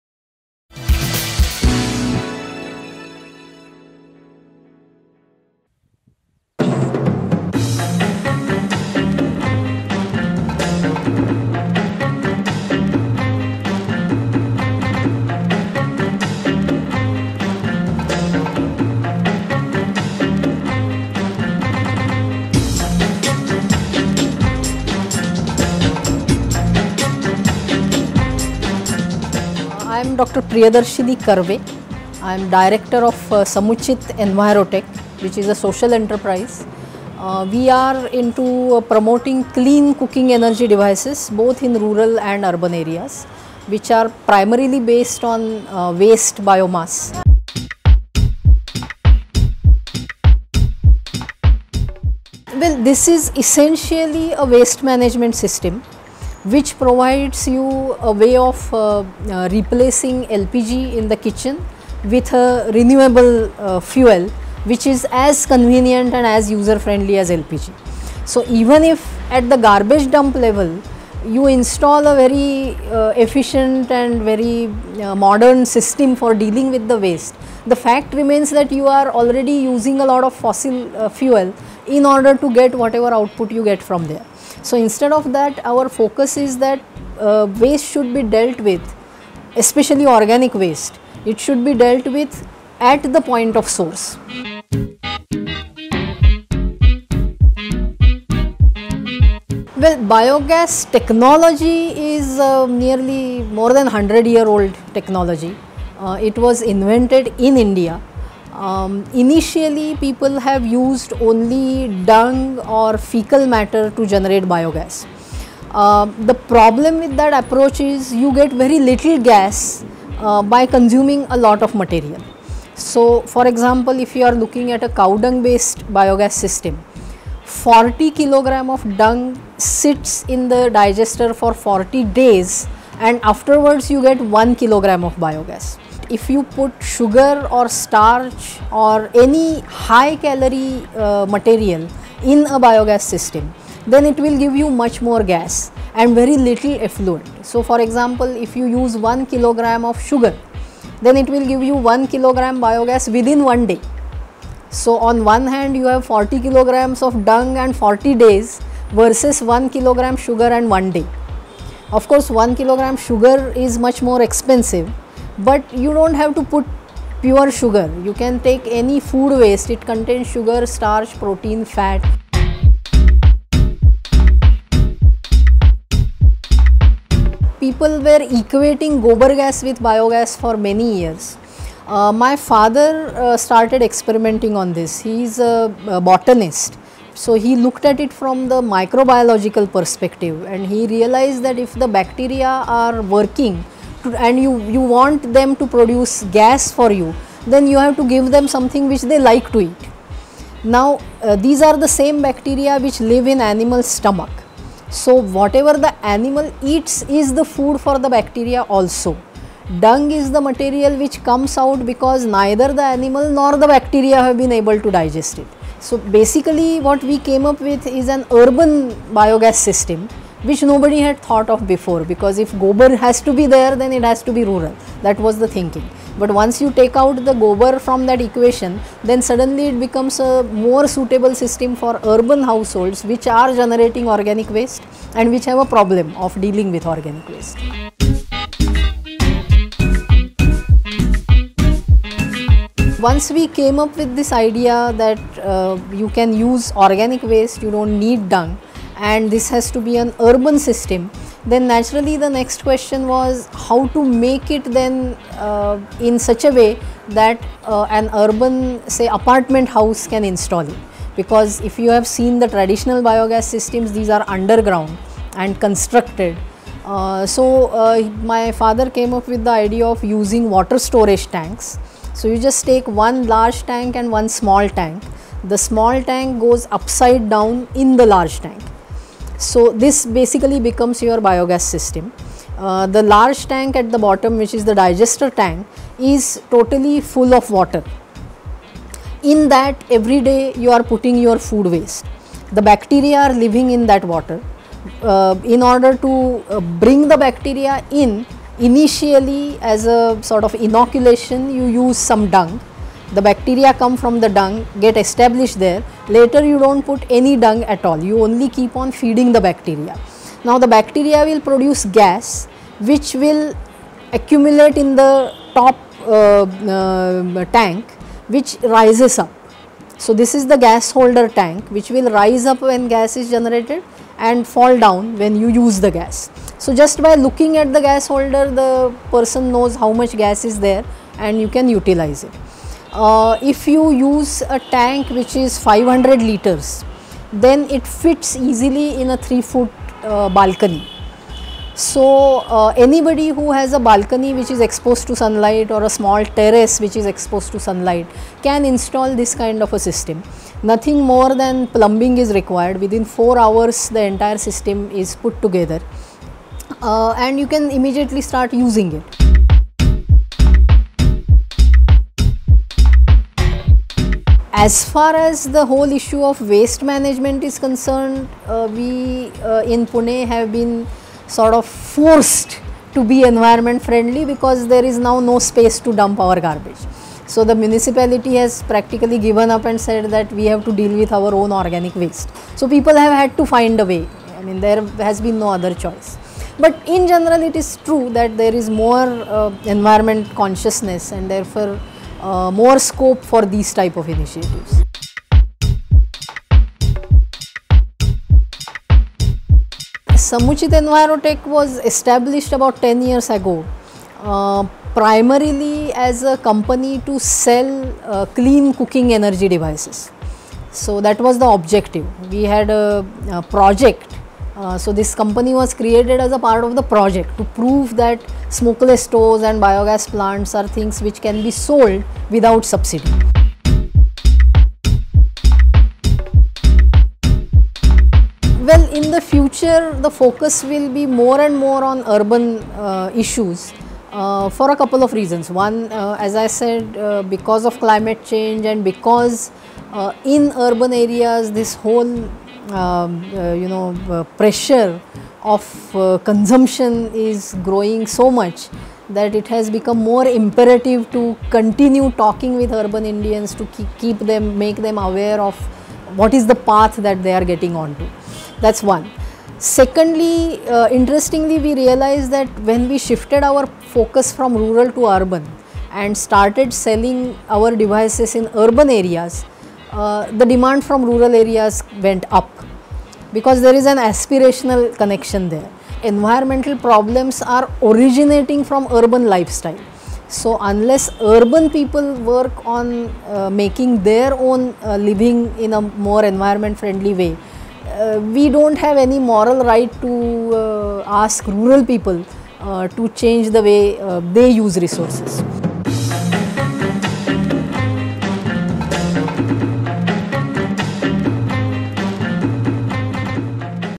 Your Very Own Home Biogas Plant: In Conversation with Dr Priyadarshini Karve
Pune today is known as one of the most environmentally conscious cities of India. Home to many people who are environmentally motivated, Pune is also where Samuchit Enviro Tech is based. This group of people are promoting the urban home bio-gas setup, something that eats up all your kitchen waste and makes bio-gas out of it.